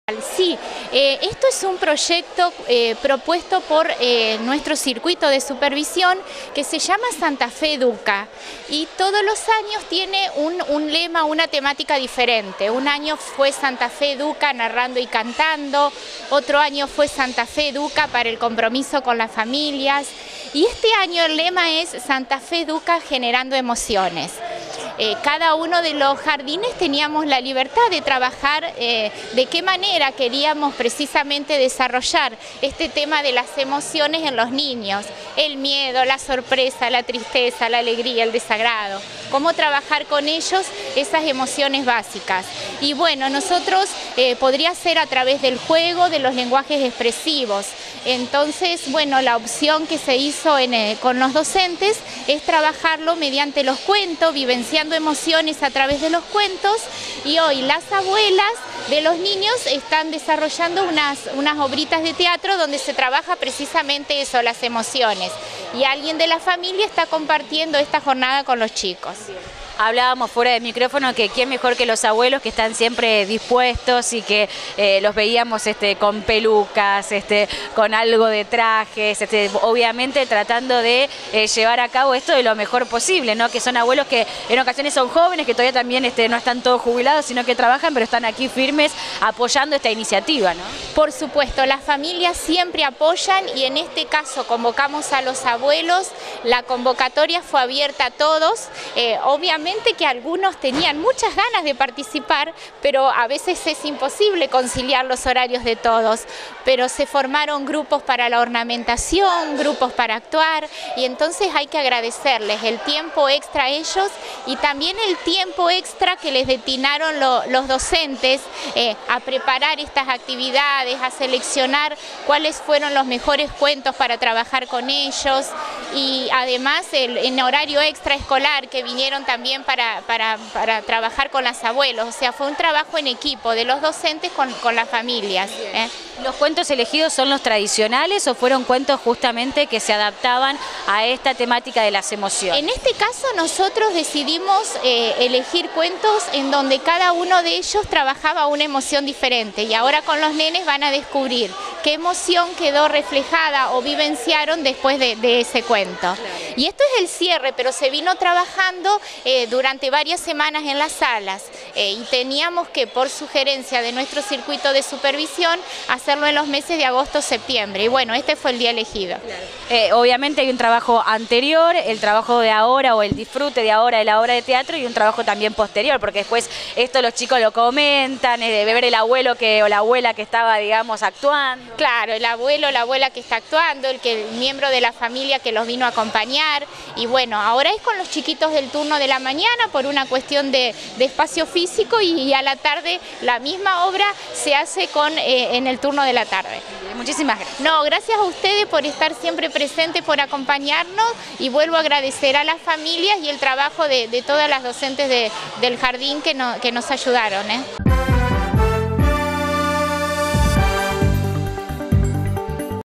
Colegio Corazón de Jesús - Proyecto "Santa Fe educa"
Los abuelos realizaron una obra de teatro para el nivel inicial